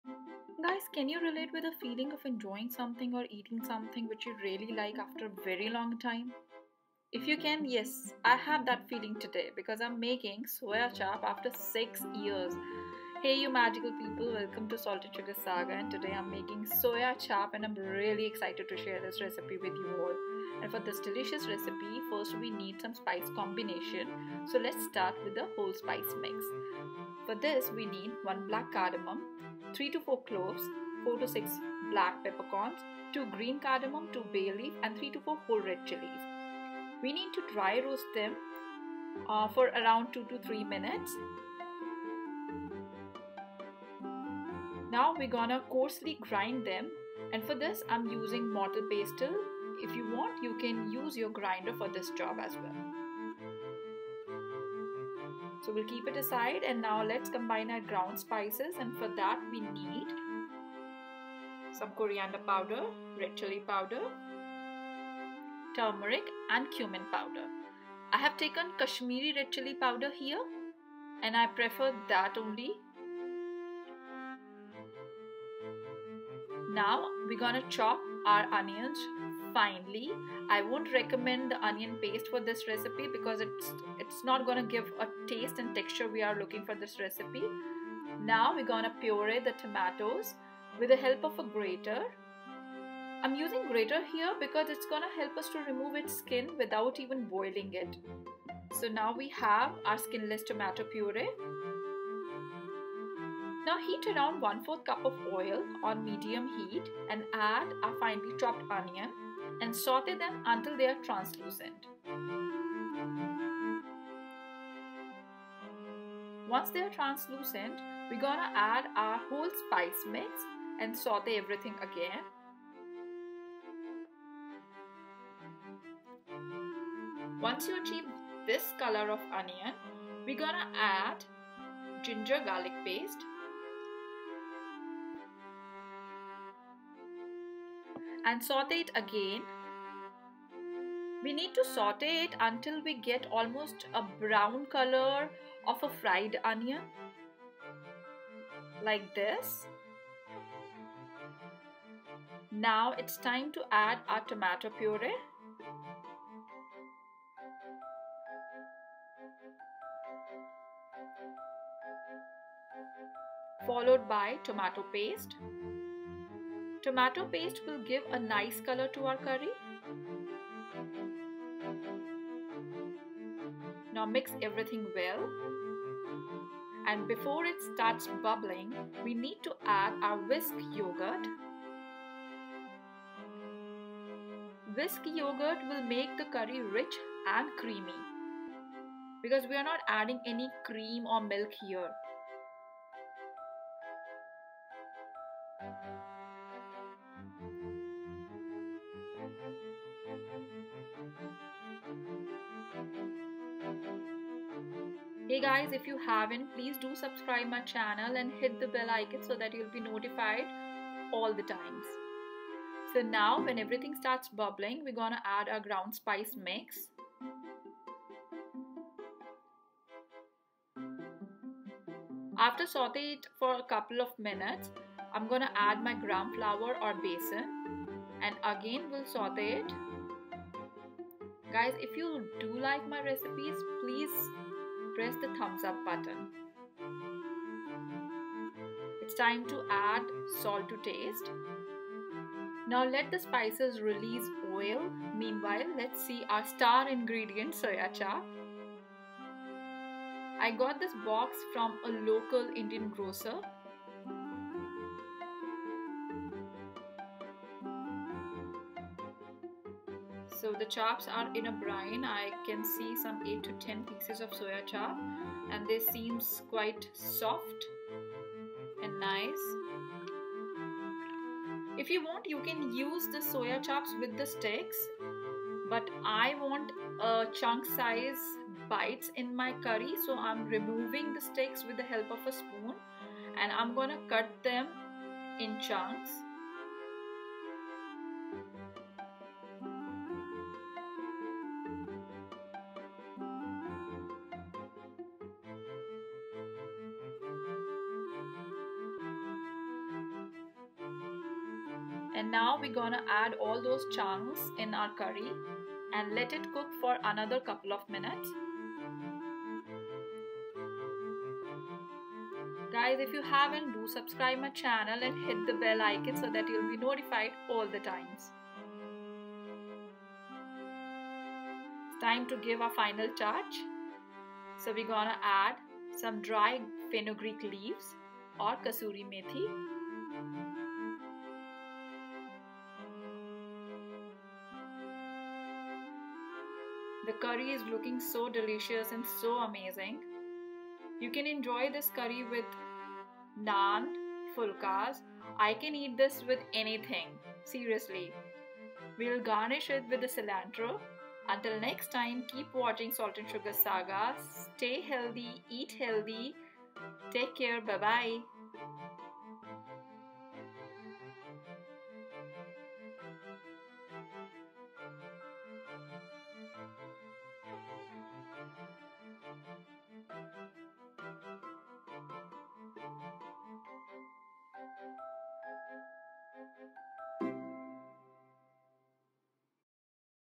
Masala Soya Chaap Curry| Dhaba Style | A taste that stays with you
Masala soya chaap curry is created in a Dhaba style recipe, it’s taste stays with you and you want to make on every possible day.
I am making this recipe almost after six years as it wasn’t available, but now am really excited to share this recipe with you all as it one of my favorite recipe.
Do share your reviews with and # cookwithsaltnsugarsaga

For more exciting recipes follow us on 👇🏼